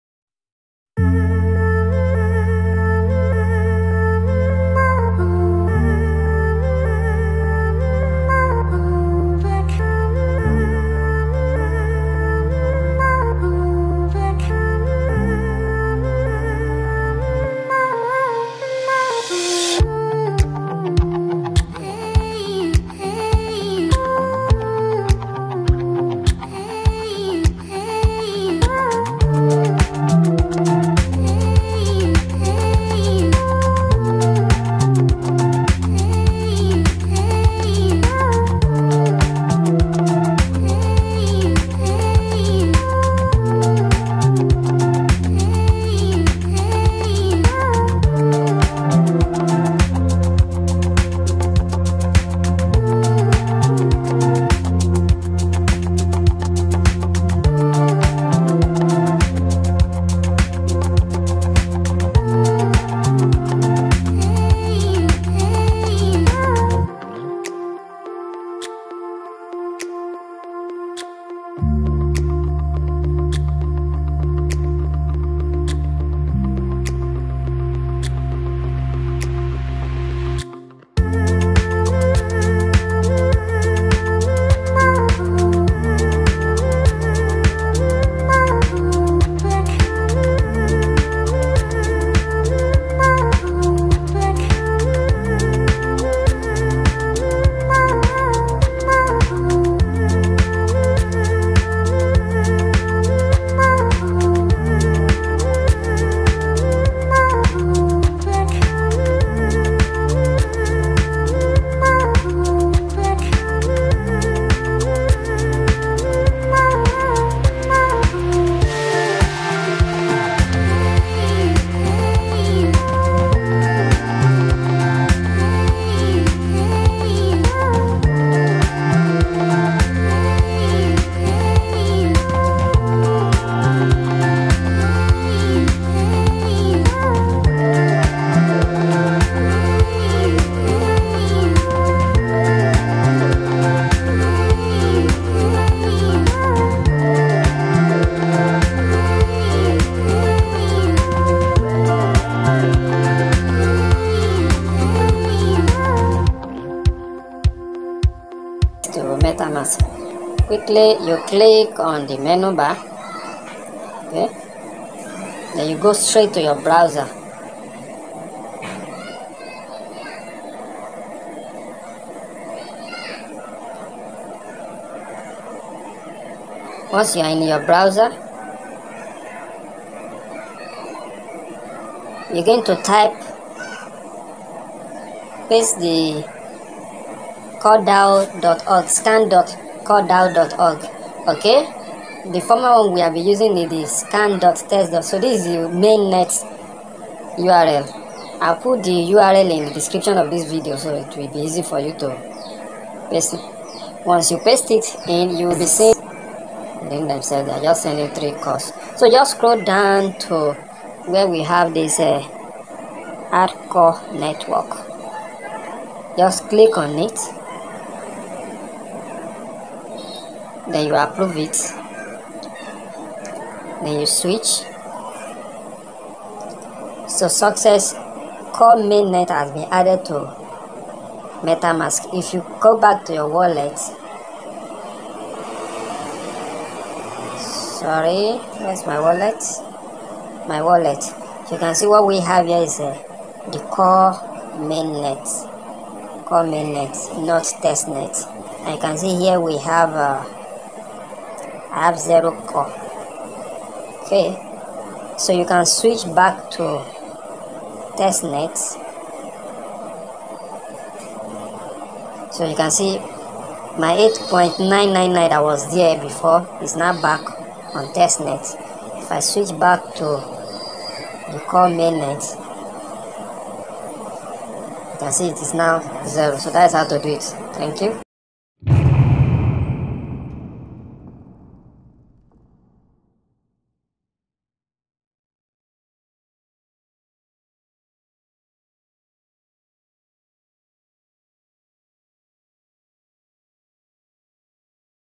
Add CORE to Metamask Wallet by 3 Ways | add core chain Mainnet in Metamask
Video info
How to add COR Mainnet on Metamask wallet
How to add CORE in metamask wallet
Metamask walletမှာ CORE Mainnet ထည့်နည်း
How to add CORE to Metamask
How to add core mainnet to Metamask
Core mainnet lunched